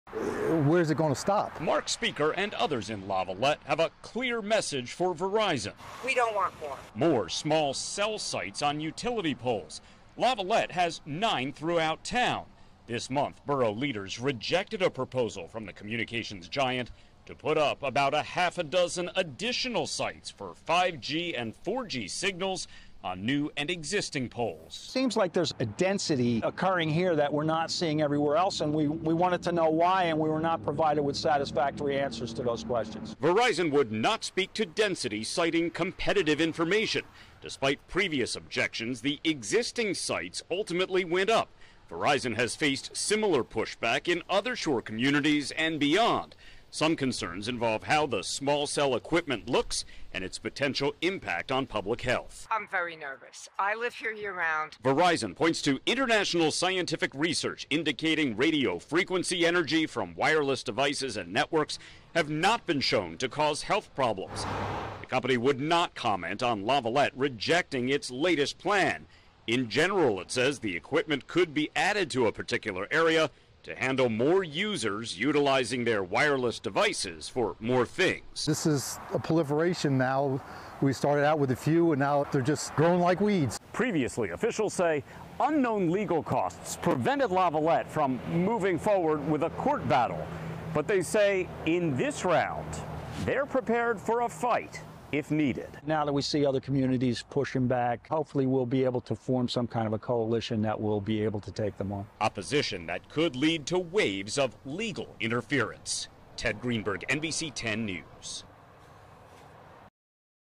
Jersey Shore town rejects Verizon's plan to add more wireless equipment on utility poles
A small Jersey Shore town is standing up to communications giant Verizon. Officials shot down a proposal to put up more wireless equipment on utility poles due to complaints that there’s already too much. NBC10’s Ted Greenberg has more on the controversy.